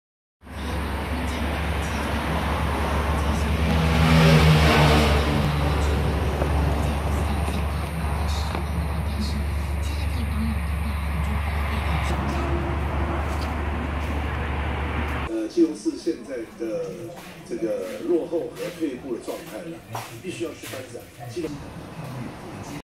HONDA CIVIC 9代進氣改裝 GIMO 能量環，喜美九代 2.0
台中 改裝進氣套件，R2車業，沙鹿阿吐，GIMO ENERGY HALO 中台灣海線區總經銷